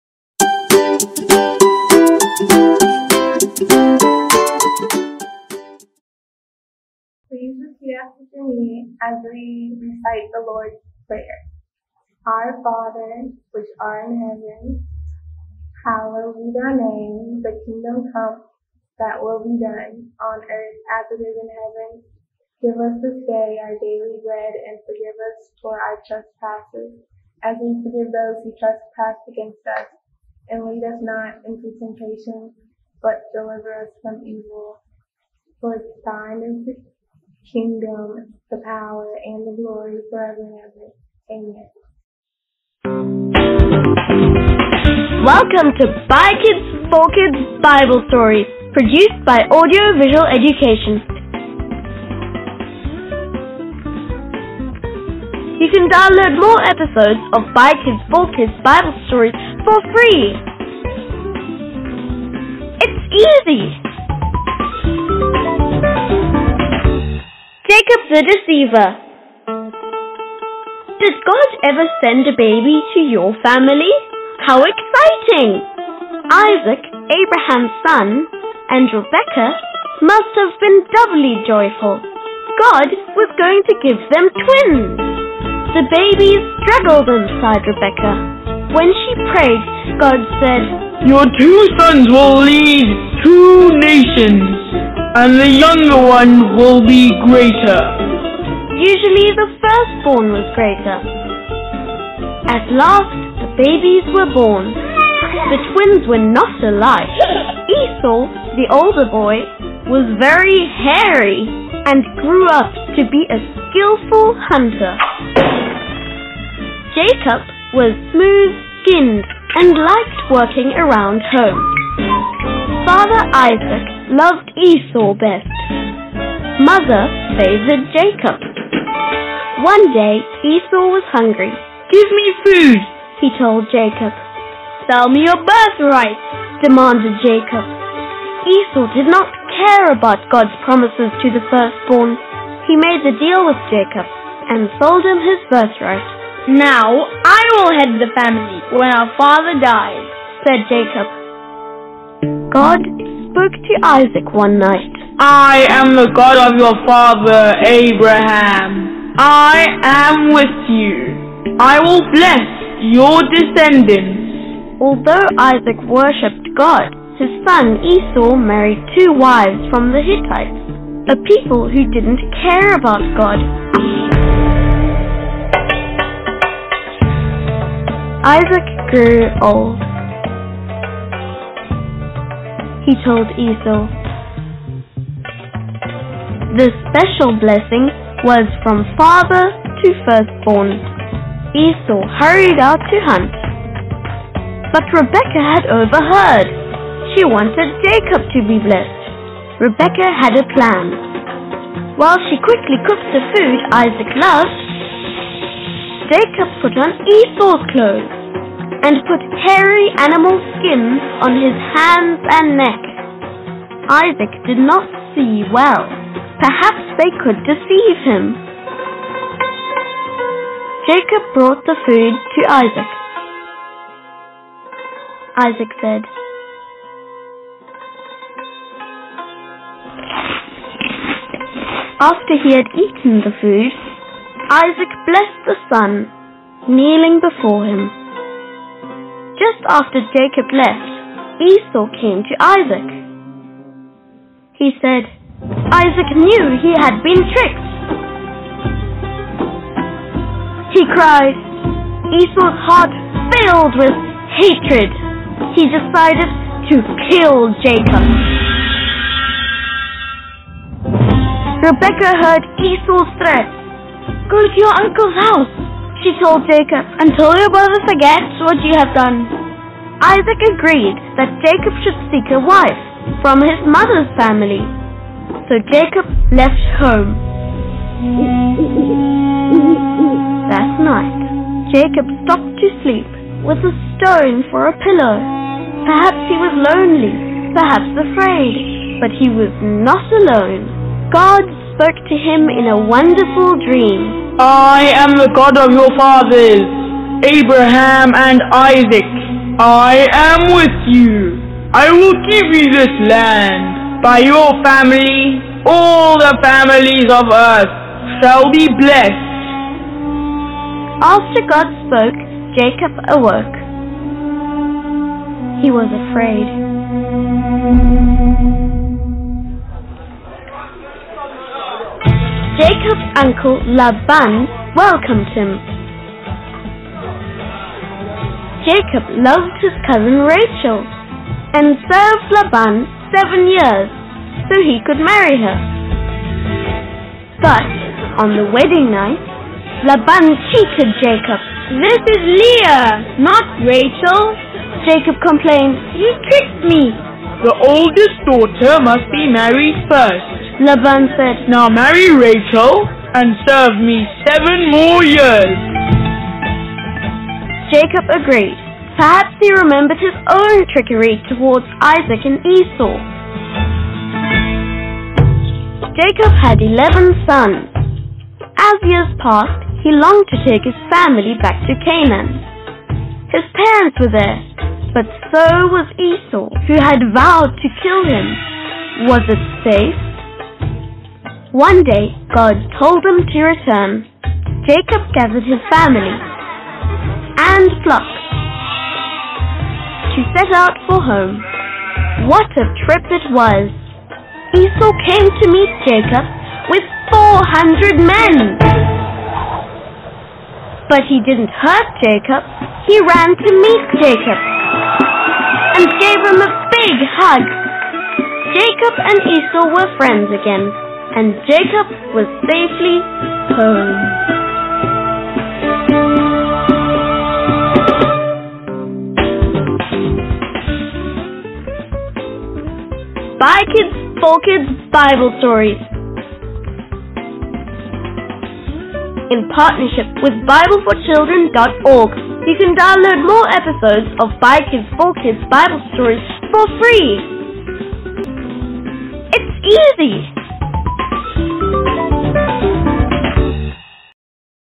Bible Hour: Jacob the Deceiver
Bible Hour Kids: Join us in devotion today and watch the story about Jacob the Deceiver. Then complete the activity and post your response to the post on our Facebook page!